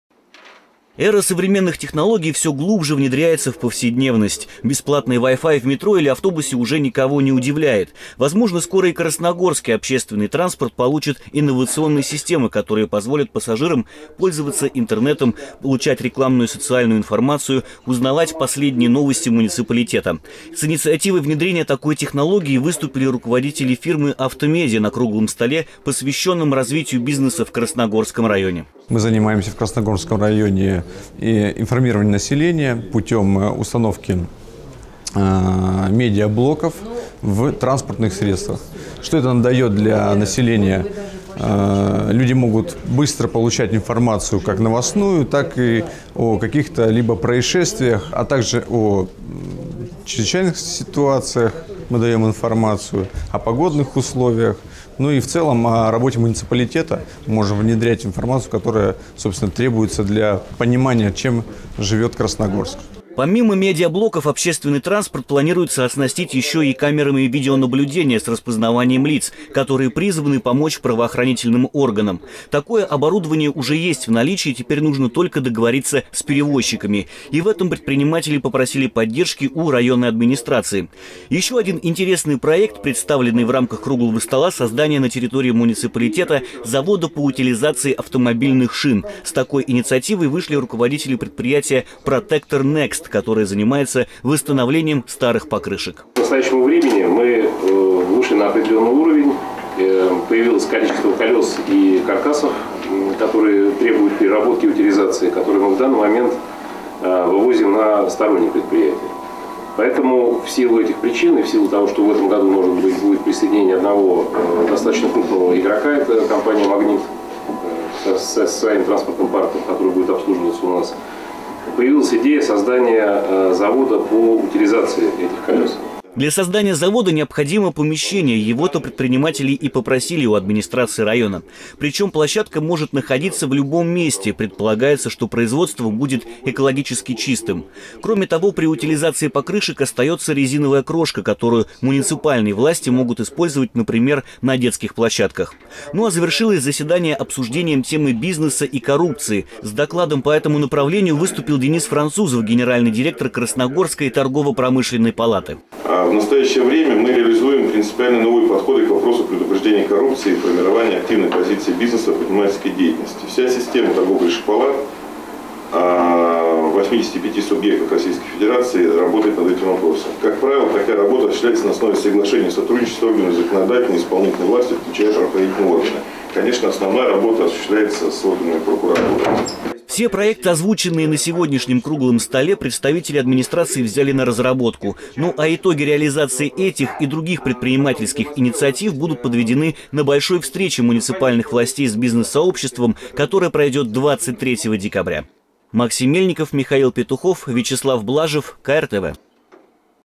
КРТВ. Муниципальные власти рассмотрят проекты бизнес-сообщества района
Система информирования пассажиров может появиться в ближайшее время в автобусах и маршрутках Красногорска. Такой проект был представлен на заседании «Круглого стола» по развитию малого и среднего предпринимательства в администрации района. Подробнее об этой и других интересных инициативах бизнес-сообщества рассказывает Максим Мельников.